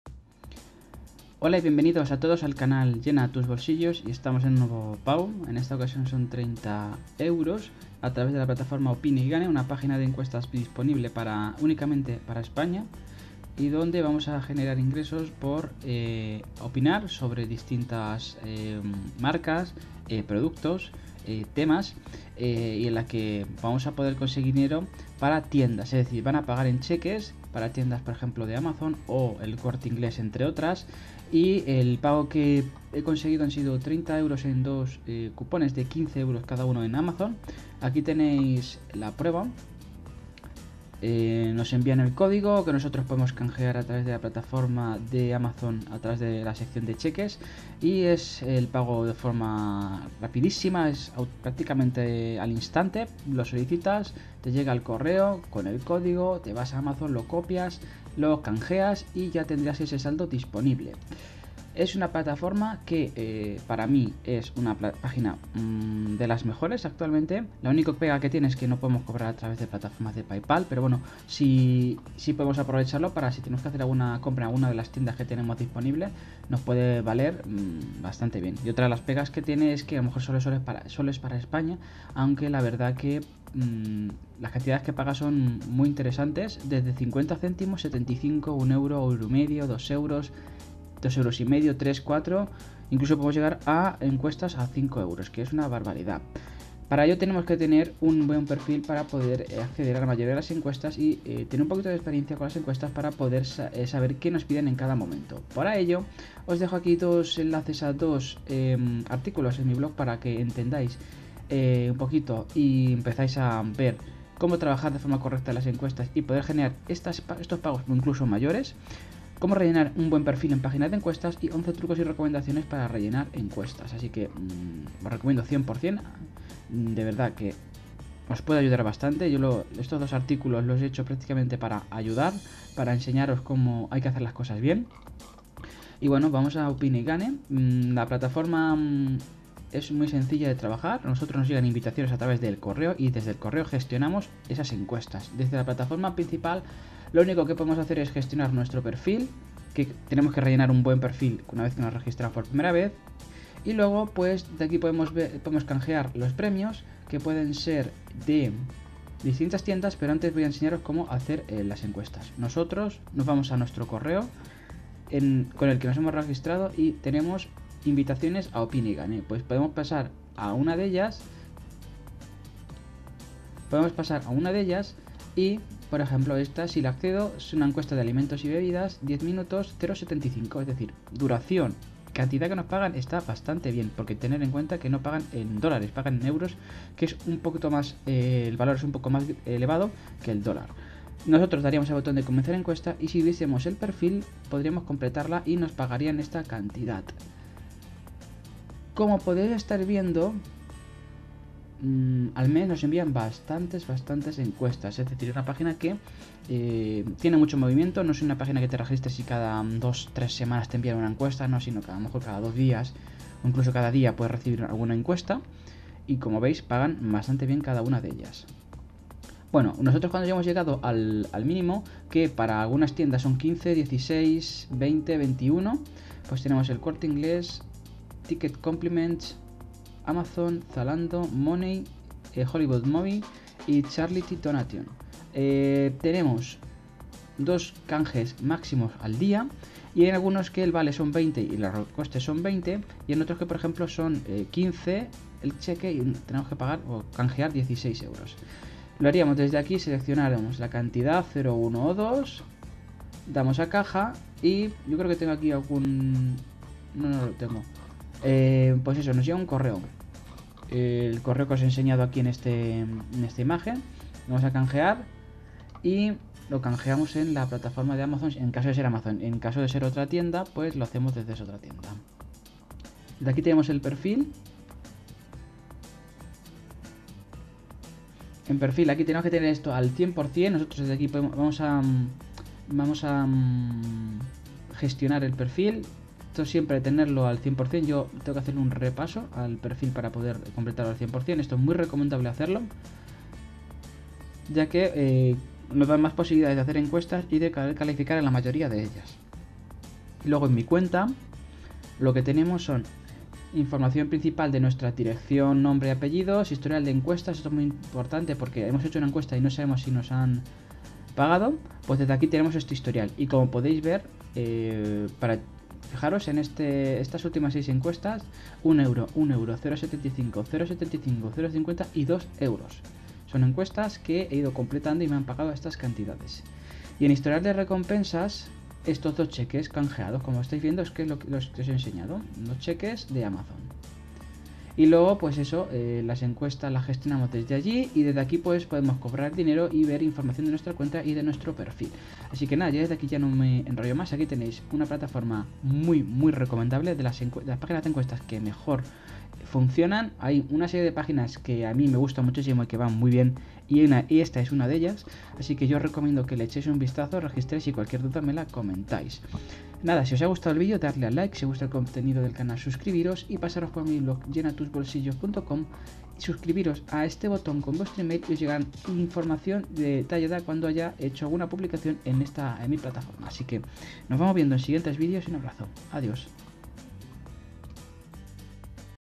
Como he ganado 30€ para Amazon Gratis | Gana dinero en internet sin inversión
🔴  NUEVO VÍDEO   🔴

Cómo he ganado 30€ para Amazon Gratis | Gana dinero en internet sin inversión

En el vídeo explico como he ganado 30€ gratis en Amazon sin pagan ni un solo Euro de mi bolsillo.

Las mejores formas de hacer dinero por internet con muchas oportunidades y opciones que tenemos al alance de nuestras manos.